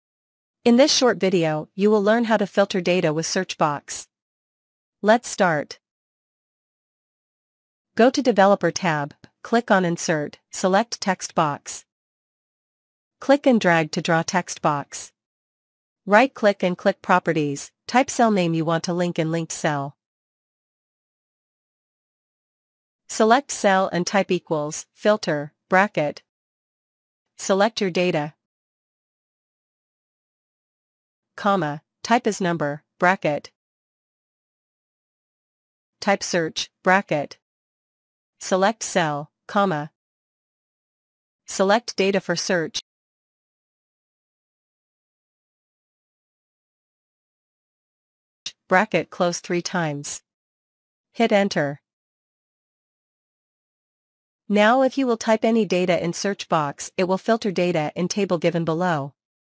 Filter data with search box in excel | How to search any entry in excel
In this short video you will learn how to filter data with search box.
Filter function in excel with is number function and search function.
How to create search box in excel.